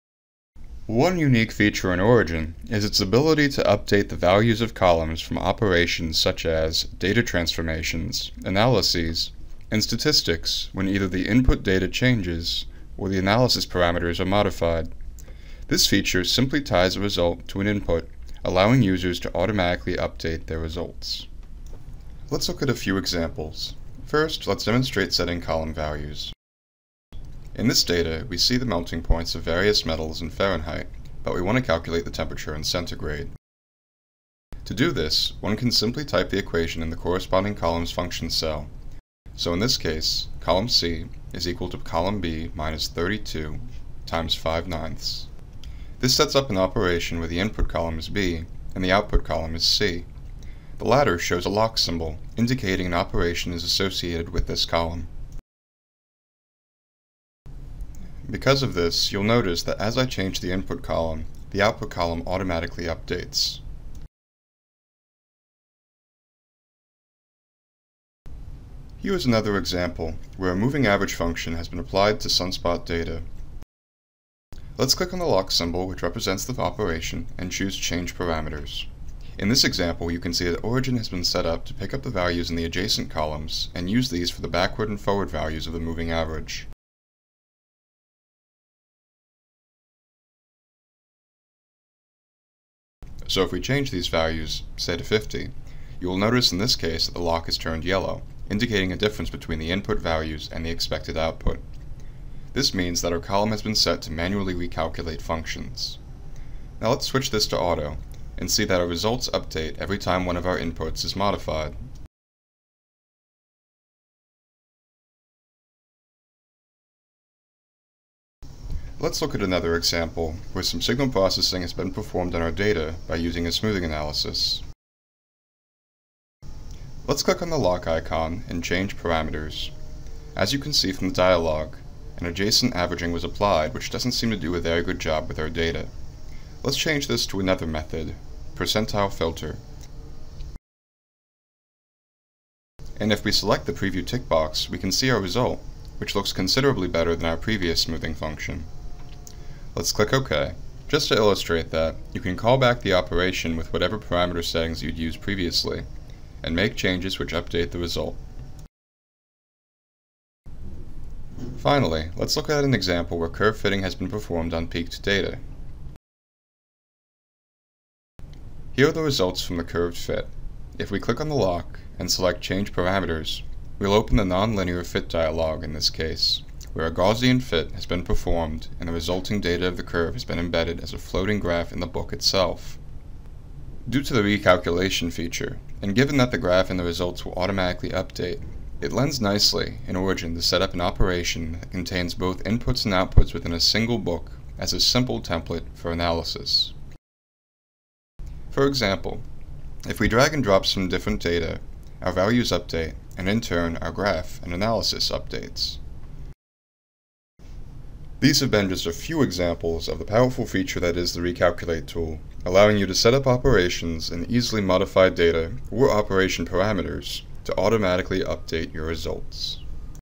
Introduction to Recalculation in Origin (VT-2568)
One unique feature in Origin is the ability to recalculate result when input data or analysis parameters are changed. This feature allows users to perform batch analysis by building a template.